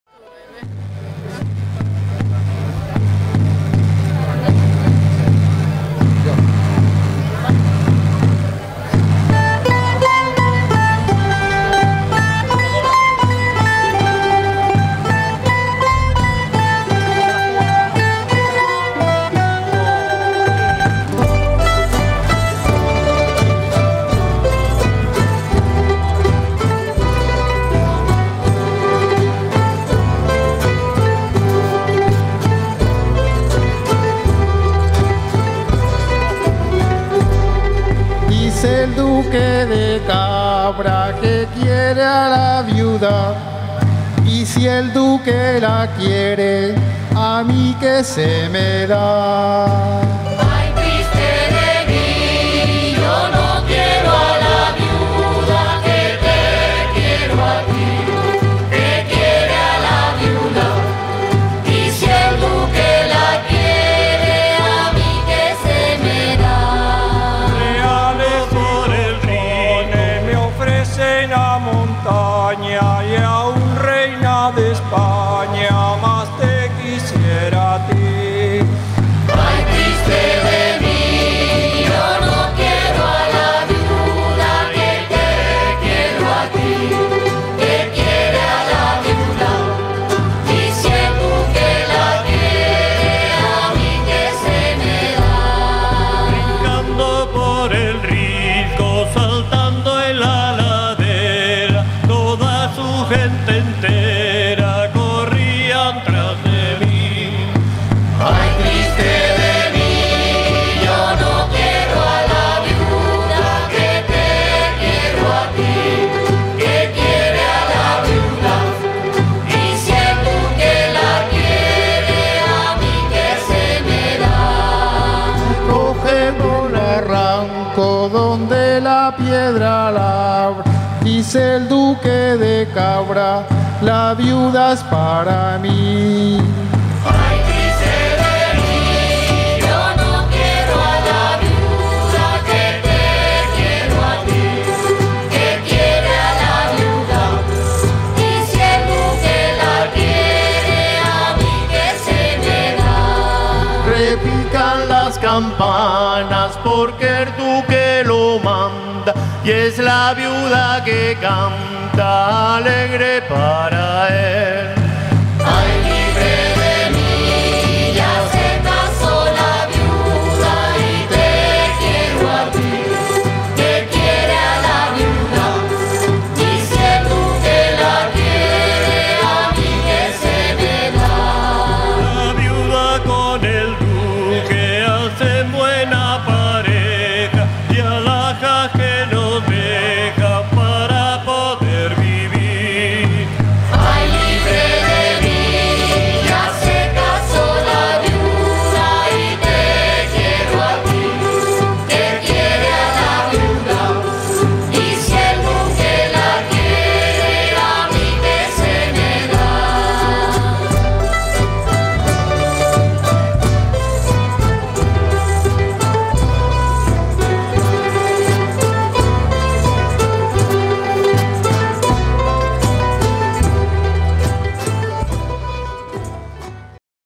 A.F. LABRANTES. ARUCAS. CON " EL DUQUE DE CABRA "
V BAILE DEL QUESO EN LA PLAZA DE GUIA. G. CANARIA. ABRIL 2016. ORG. ESTRELLA Y GUIA. PARTICIPAN 27 CUERPOS DE BAILE.